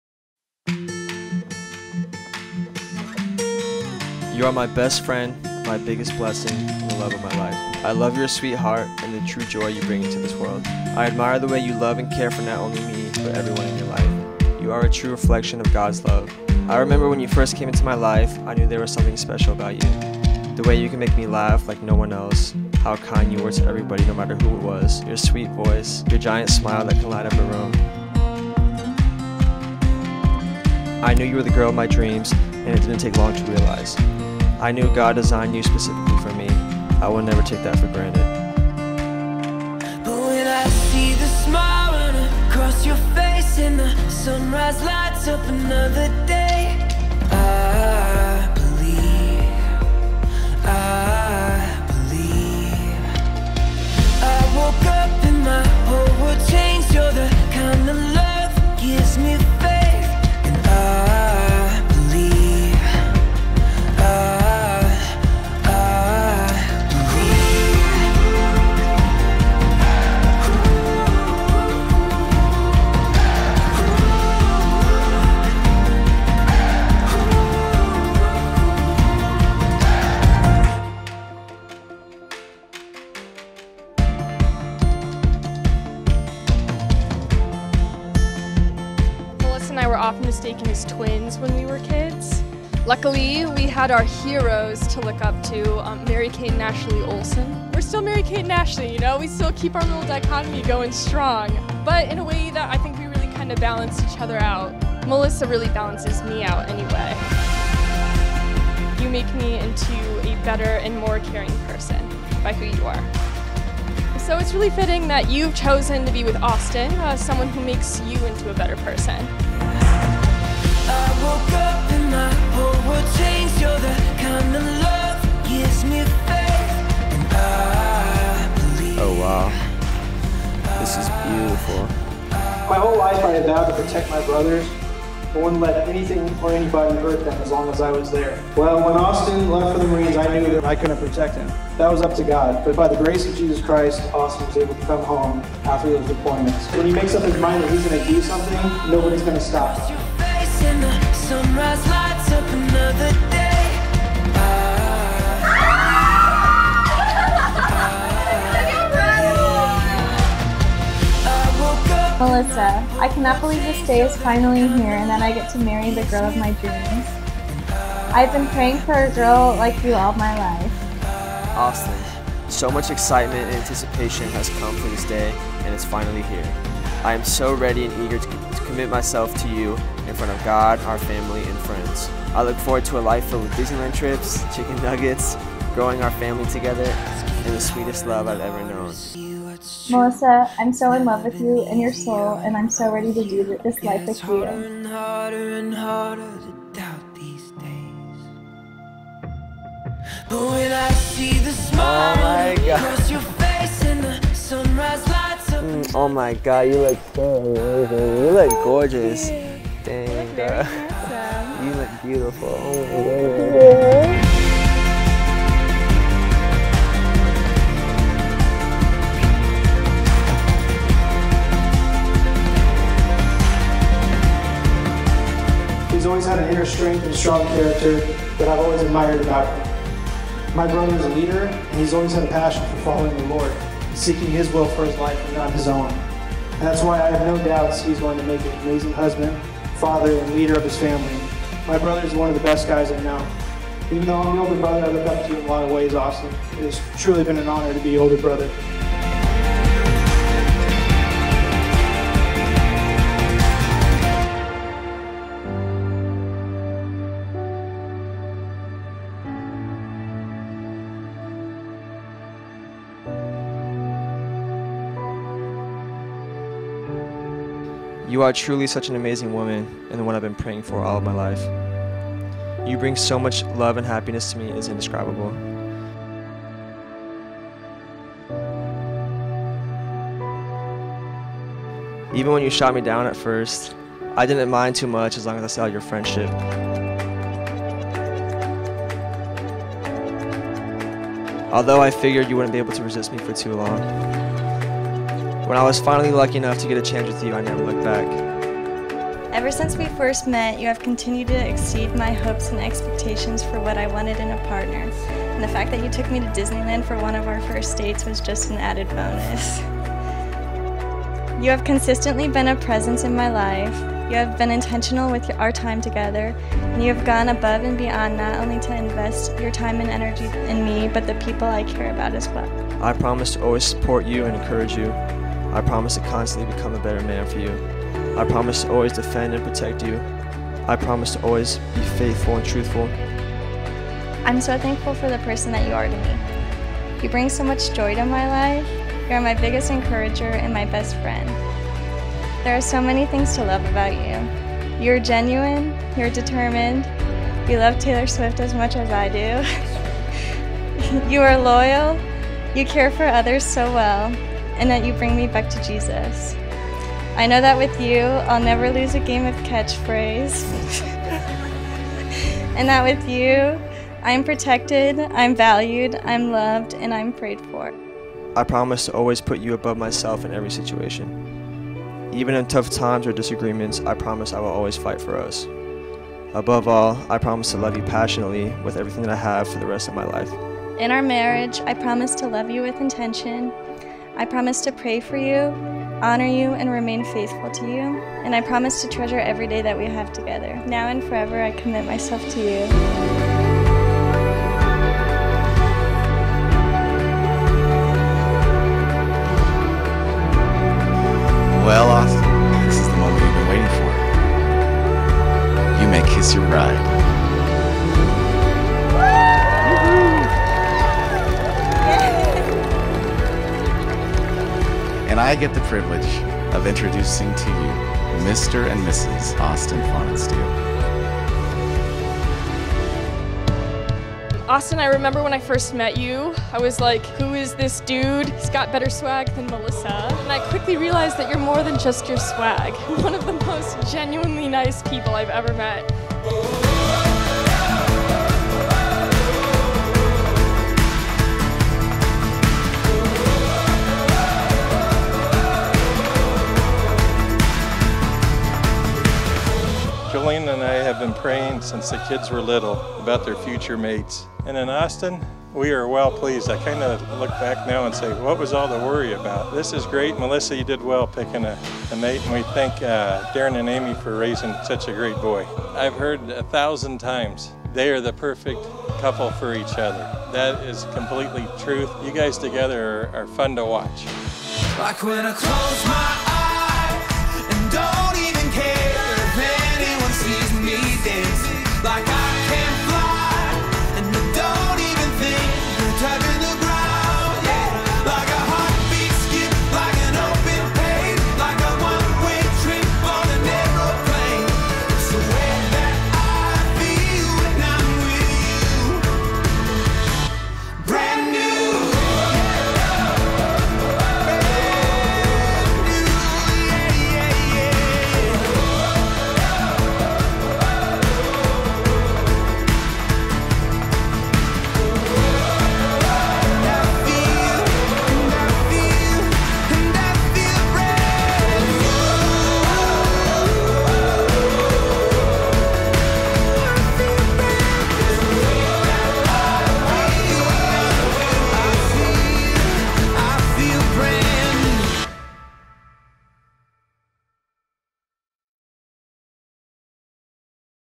Their Reception Dance Moves Will Make You So Happy...We Promise!
The bride and groom have the most fun dance moves and we were just smiling the entire time we edited this film. Their love for one another is so sweet and their wedding was beyond gorgeous at Blackstone Country Club in Peoria, Arizona.